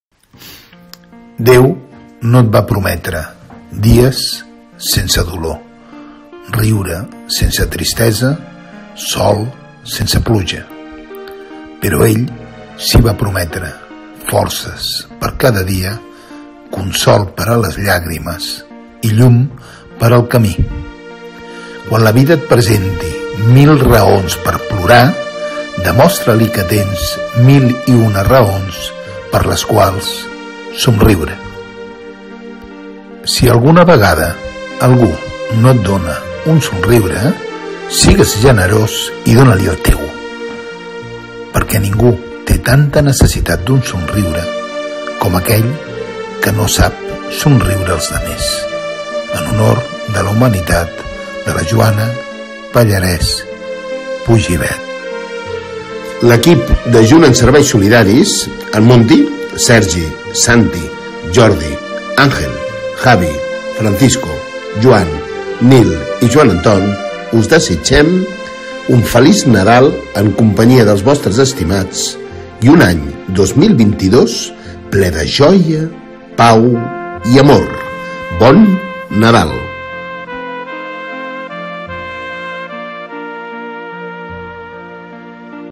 L’equip de Junan Serveis Solidaris us desitgem un Feliç Nadal
Déu no et va prometre
dies sense dolor…
riure sense tristesa
Sol sense pluja..
Però ell si va prometre
Forces per a cada dia
Consol per a les llàgrimes
I llum per al camÍ
Quan la vida et presenti
Mil raons pere plorar, demostra-li que tens
Mil i una raons per les quals SOMRIURE

. Si alguna vegada algú no et dona un somriure,
Sigues generós i dóna-li el teu,
Perquè ningú té tanta necessitat d’un somriure
Com aquell que no sap somriure als demés
En honor de la humanitat de la Joana Pallarés Puigibet

L’equip de Junan Serveis Solidaris, en Monty, Sergi, Santi, Jordi, Angel, Javi,Francisco,  Joan, Nil i Joan Antón, us desitgem Un Feliç Nadal en companyia dels vostres estimats i un Any 2022 ple de joia , pau i amor
BON NADAL